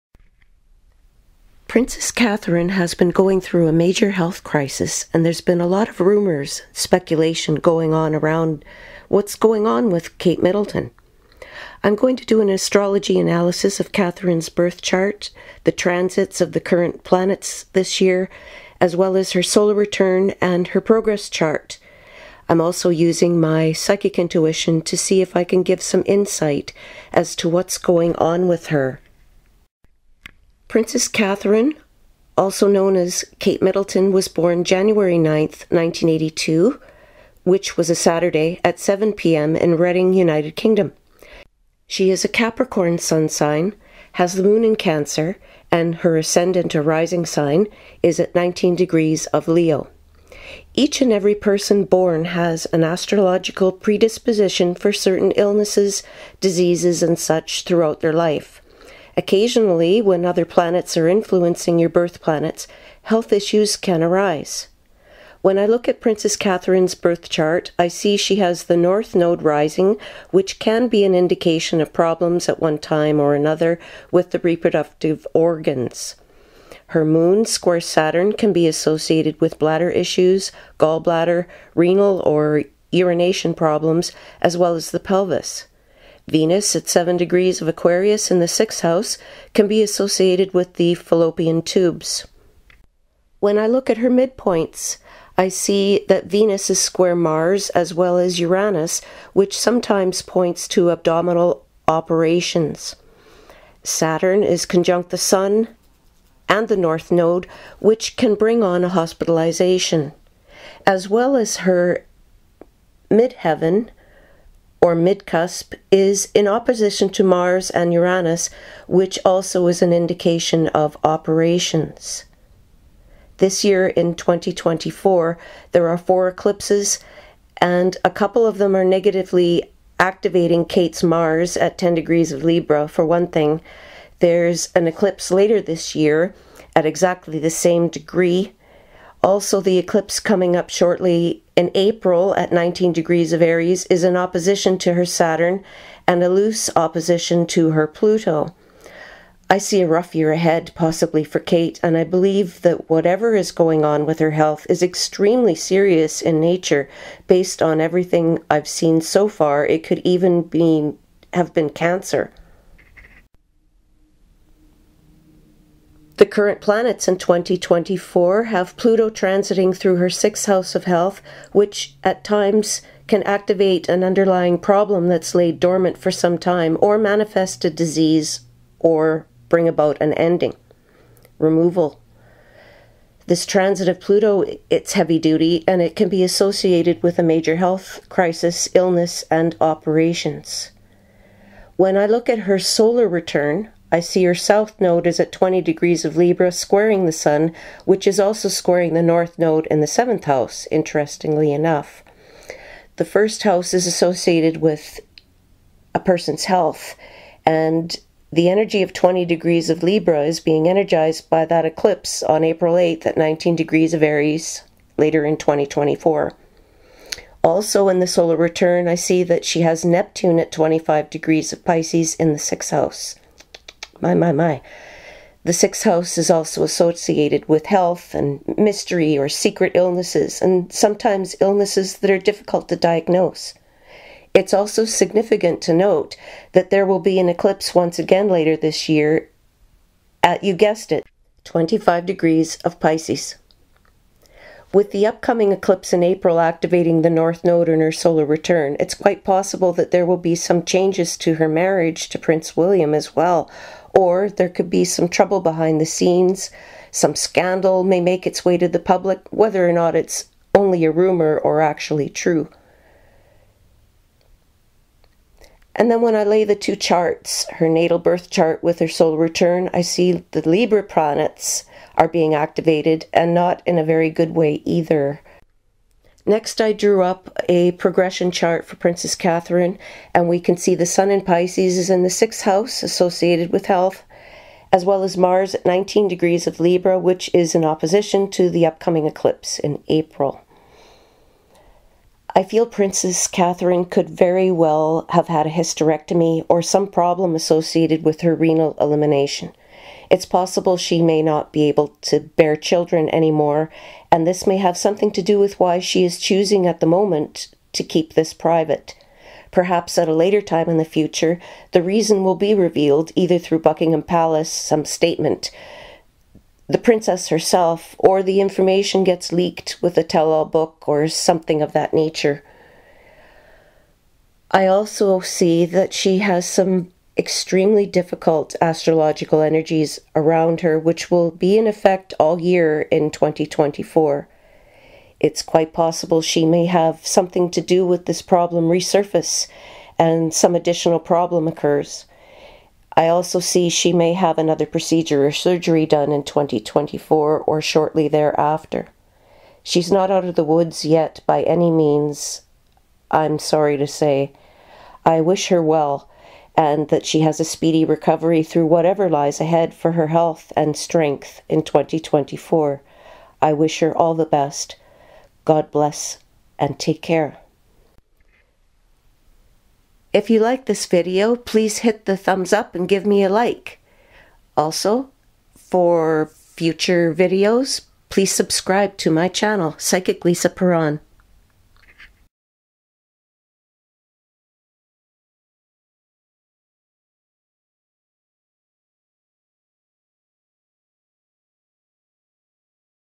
What is going on with Princess Kate? Psychic Astrology Reading
What is going on with Princess Kate? Psychic and Astrological Reading for Princess Kate. Princess Catharine has been going through a major health crisis and there’s been a lot of rumors and speculation going around about what’s going on with Kate Middleton. I’m going to do an astrology analysis of Catherine’s birth chart, the transits of the current planets this year, her solar return, and her progress chart as well as my psychic intuition to see if I can give some insight as to what’s going on with her. Predictions for the Future by Psychic Lisa Paron.

          PERSONAL READINGS WITH PSYCHIC LISA PARON
                                    ASTRO MOJO